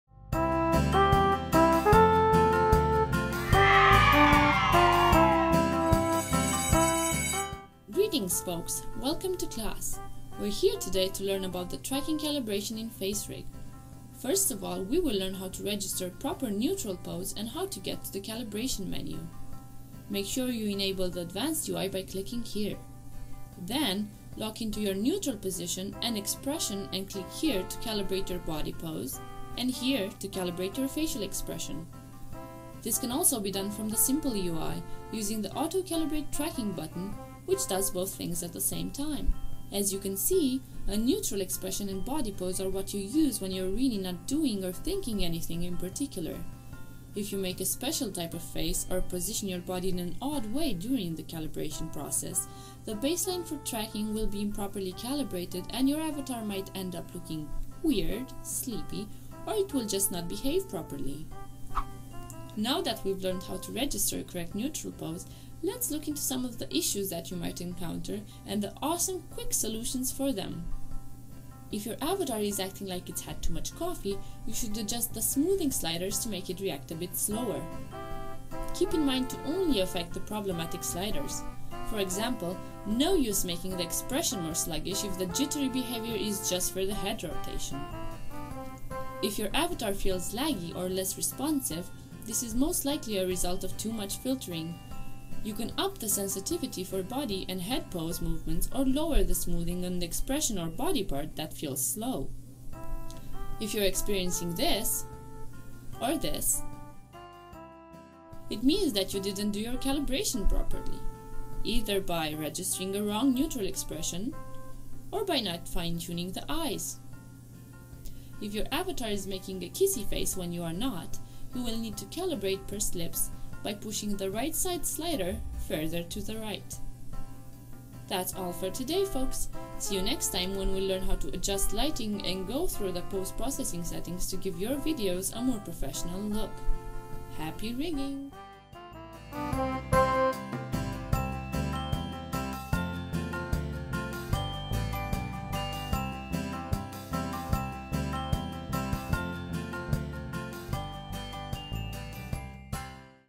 Tutorial: Calibration
This short tutorial will teach you how to properly calibrate FaceRig:)